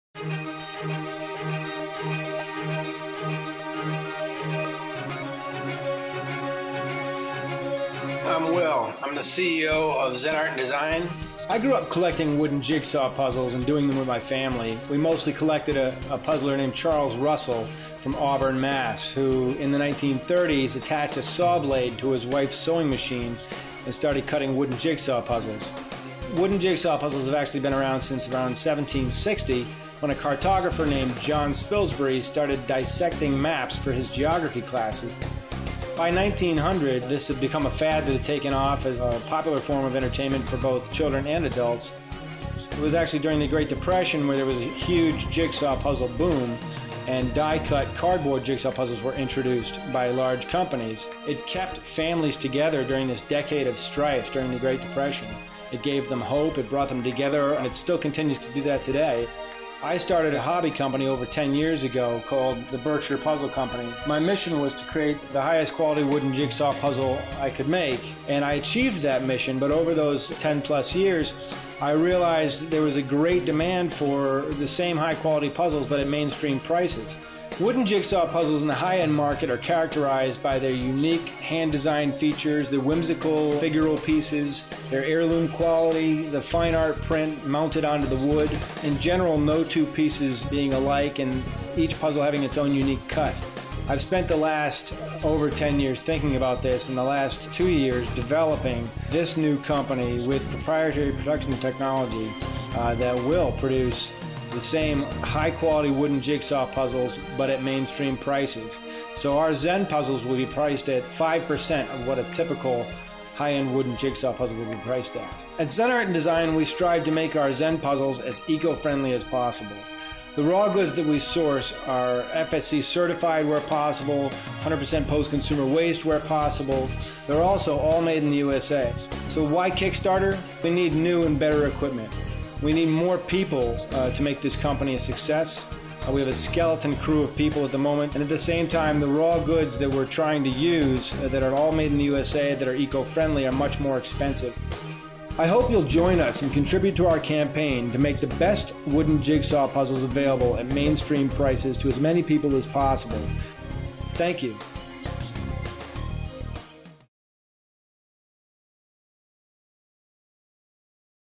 Zen Puzzles™: Artisanal, Wooden Jigsaw Puzzles Kickstarter Video
Zen Puzzles are hand-designed, artisanal, heirloom quality, wooden jigsaw puzzles, each skillfully crafted with beautiful images, unique and elegant designs, and wonderful, whimsical figural pieces, such as a lion, a jumping horse, or a couple ballroom dancing. Zen Puzzles has evolved out of the founder's 10+ years of creating some of the world's highest quality, collectible, handcrafted, wooden jigsaw puzzles for the niche, international, premium wooden jigsaw puzzle market, where prices range from $800 to $10,000 per puzzle.